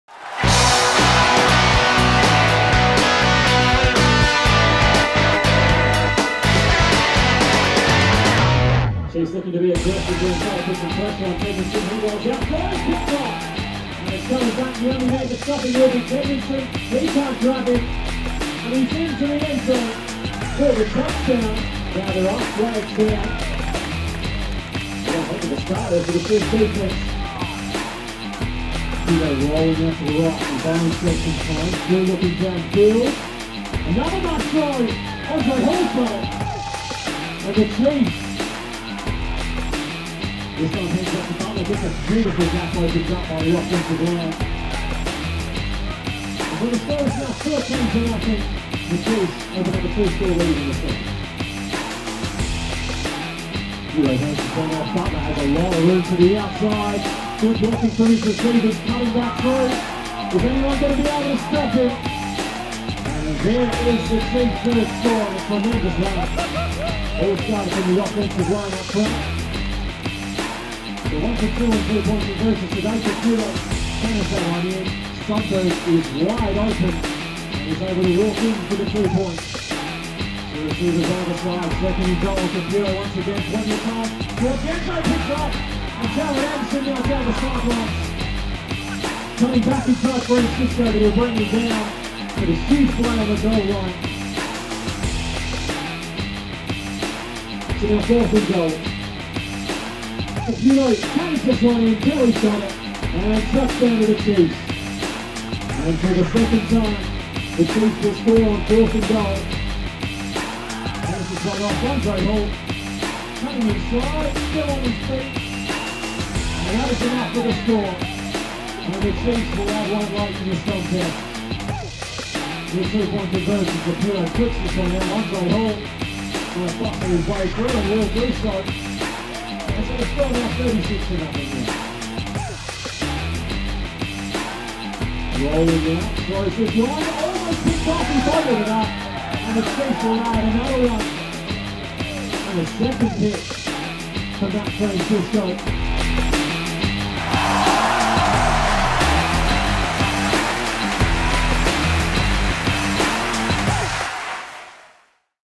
Week 6 2016, Chiefs V Oilers
South City controlled the game from start to finish, Francisco returning an INT for 6 on the second play from scrimmage. Windy conditions made throwing the ball difficult all day, the Oilers managing to finish the first half with an endzone INT, their best play of the first half.

Things didn’t improve for the Oilers in the Second, the Chiefs continued to move the ball, adding multiple scores in the third. Late in the fourth the Oilers replaced starter Tedmanson at QB, his replacement giving Francisco his second INT return for a TD to finish the scoring.